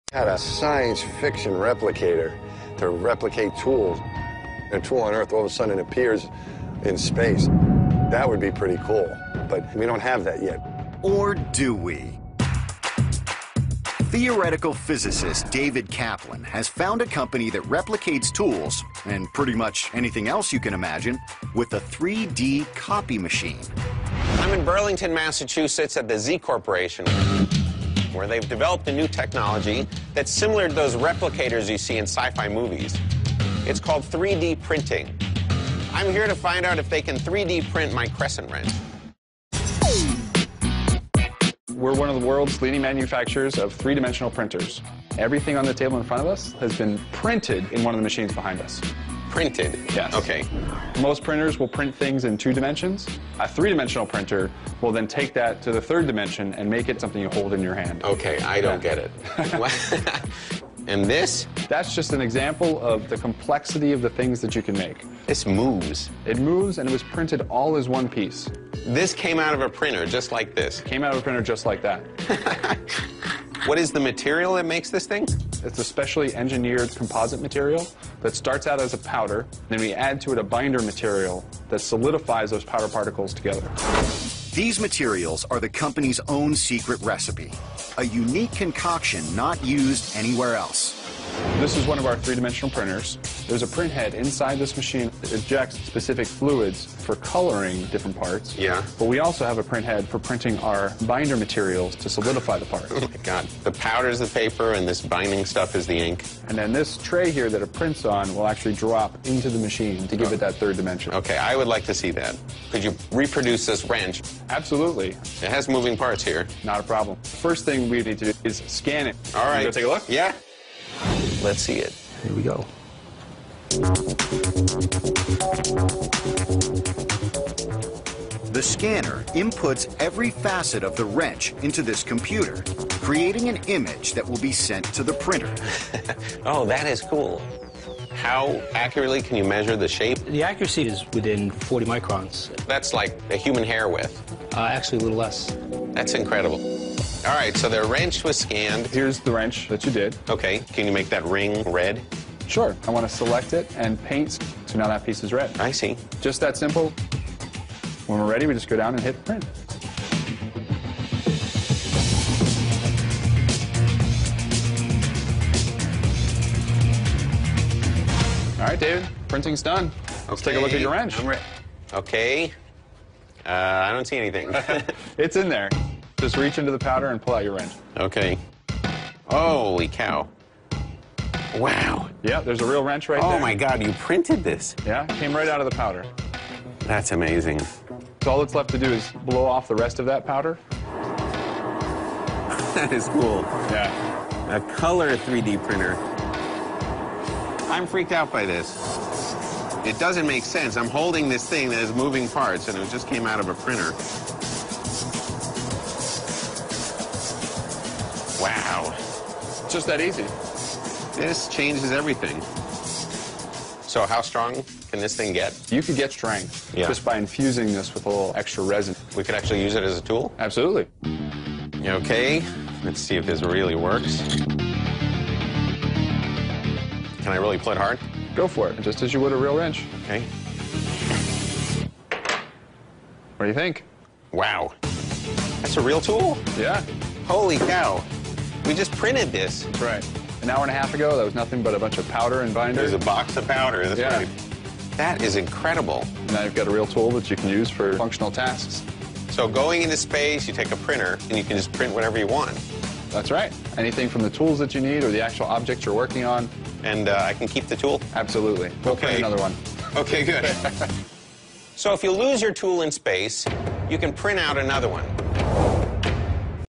Amazing Technology - 3D Printer !!!
Imagine the Possibilities !

3D printing is a form of additive manufacturing technology where a three dimensional object is created by laying down successive layers of material. 3D printers are generally faster, more affordable and easier to use than other additive manufacturing technologies. 3D printers offer product developers the ability to print parts and assemblies made of several materials with different mechanical and physical properties in a single build process. Advanced 3D printing technologies yield models that can serve as product prototypes.